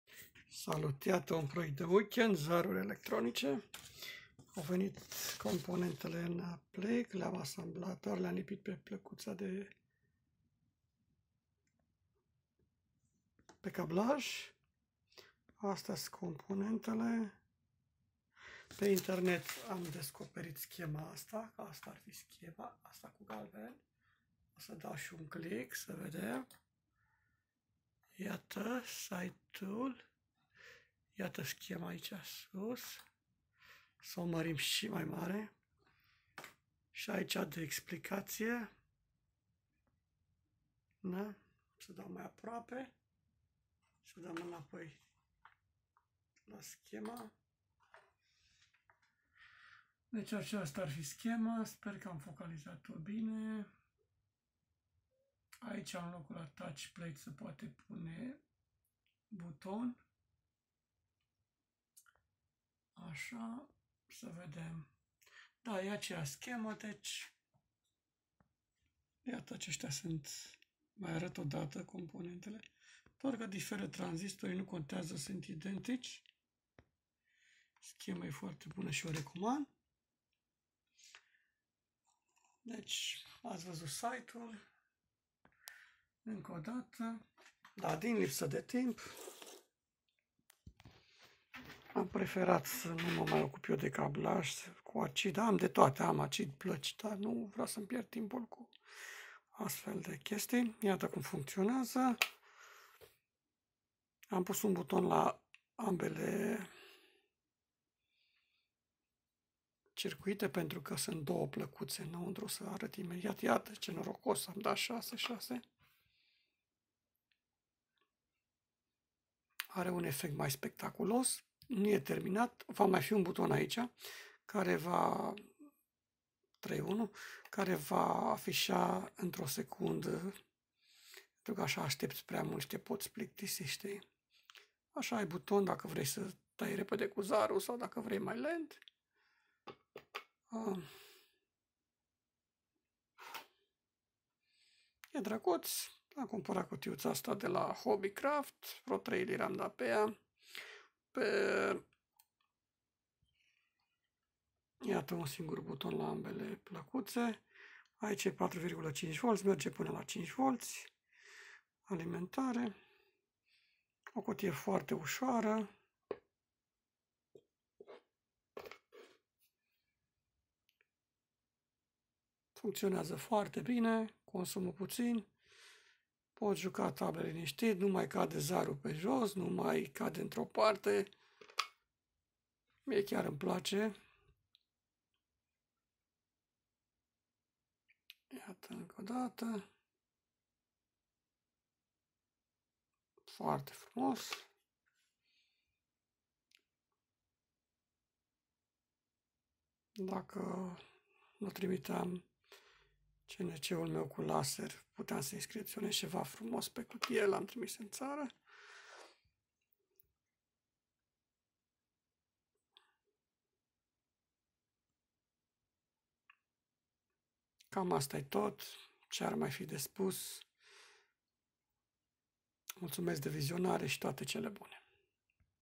Zaruri electronice Electronic Dice
Am cumparat doua kituri pret total 5 .5 lire sterline doar am lipit piesele pe placuta.Placuta e inscriptionata si e floare la ureche de asamblat.